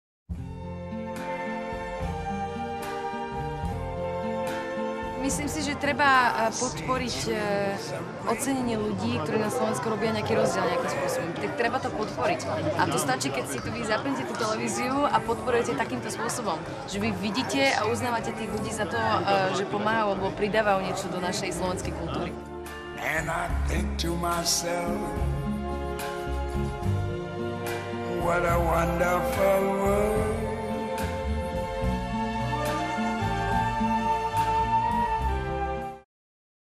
Celeste Buckingham: Krištáľové krídlo podporuje ľudí, ktorí sú prínosom
Krištáľové krídlo hľadá tých najlepších medzi výnimočnými. Hľadáme osobnosti, ktoré v roku 2016 zaznamenali výnimočné počiny. Do 31. októbra môžete svojho favorita navrhnúť aj Vy. Stačí vyplniť online formulár na